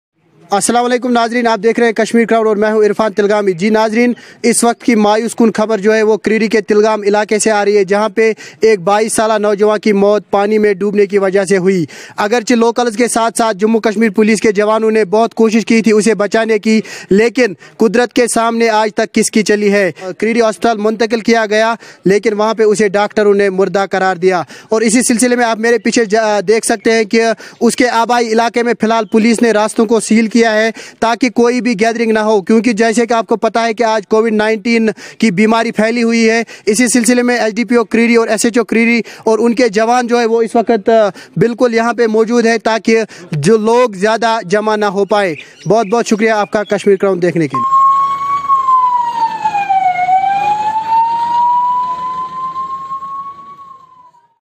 Twenty Two Year Old Boy Drowned Into Nallah At Frasthar Kreeri
Report Irfan Tilgami